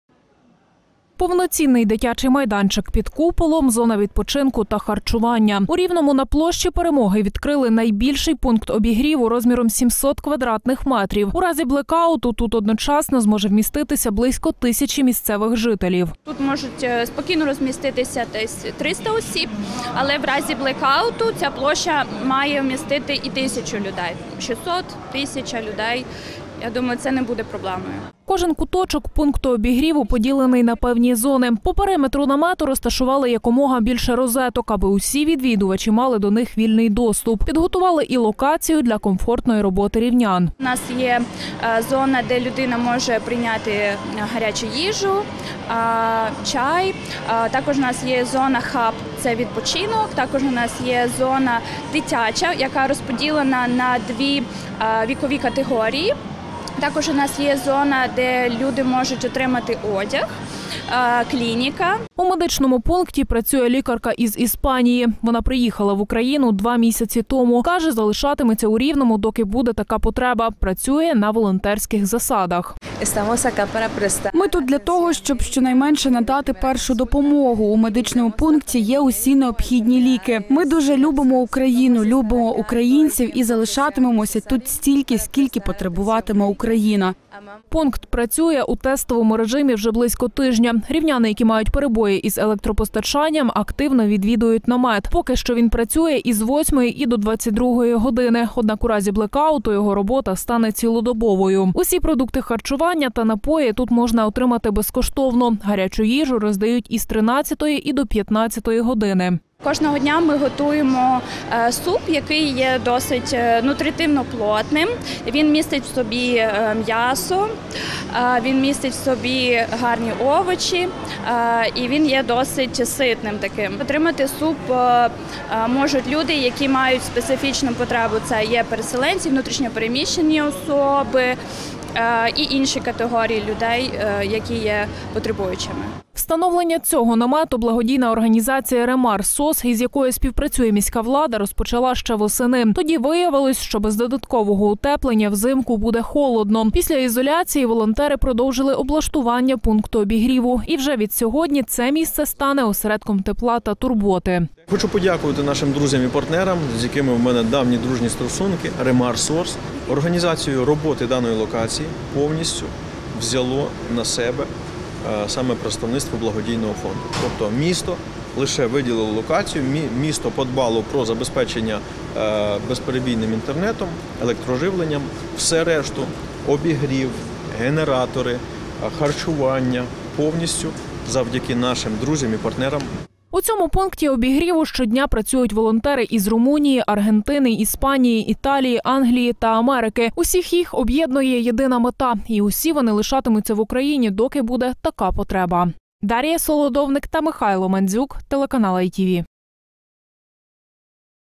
Найбільший пункт обігріву в Україні відкрили у Рівному
Найбільший пункт обігріву. І не лише на Рівненщині, а й в Україні. У обласному центрі відкрили локацію, у якій одночасно може знаходитися тисяча рівнян. Поки що це єдиний пункт обігріву у якому можна отримати ще й гаряче харчування. Де знаходиться ця локація, як працюватиме намет та чим дивуватимуть рівнян, у матеріалі Дарії Солодовник.